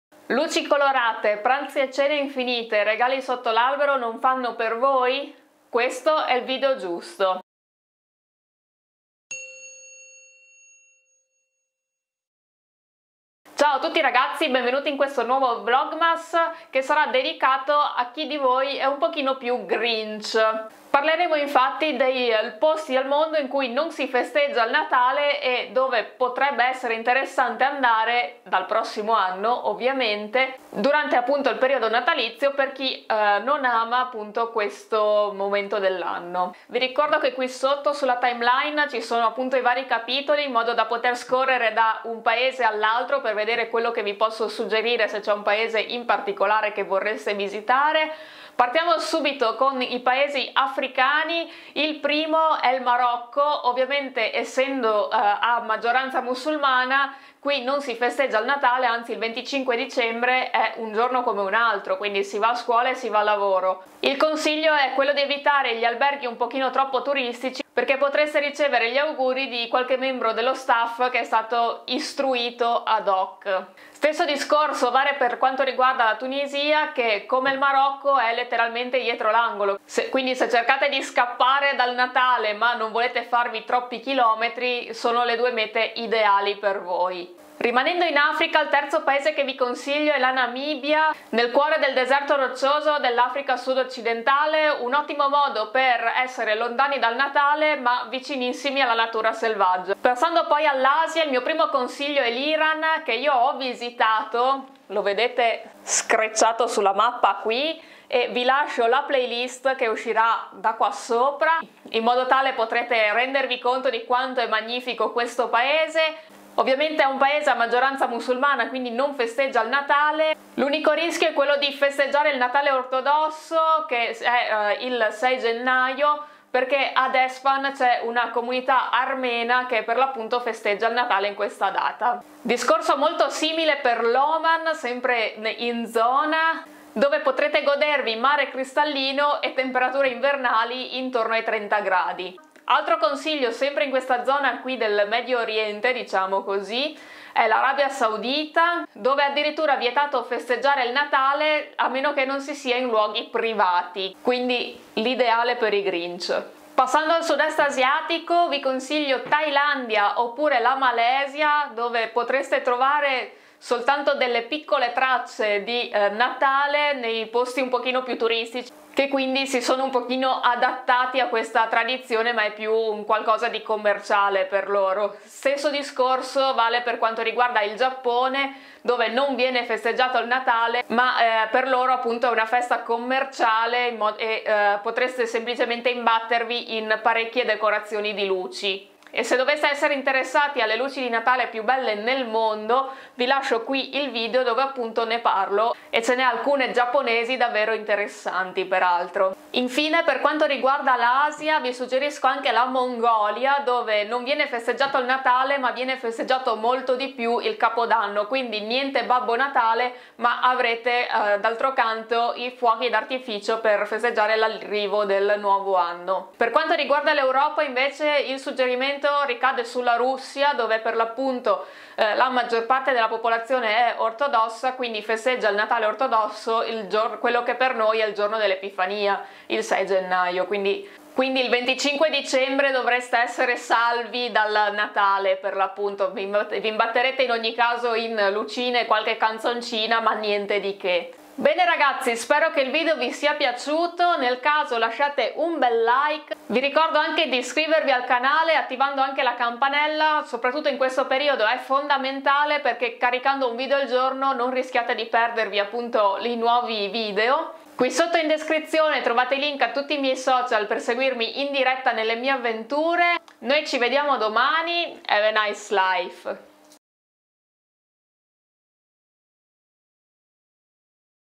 Le migliori mete per i Grinch che vogliono scappare dal Natale 💚 VLOGMAS DAY20
Dopo tanti video dedicati al Natale, ecco un Vlogmas con le migliori mete per i Grinch che vogliono scappare dallo spirito natalizio del periodo.
00:00 - Introduzione
00:48 - Africa
01:39 - Asia
03:39 - Europa
04:01 - Conclusione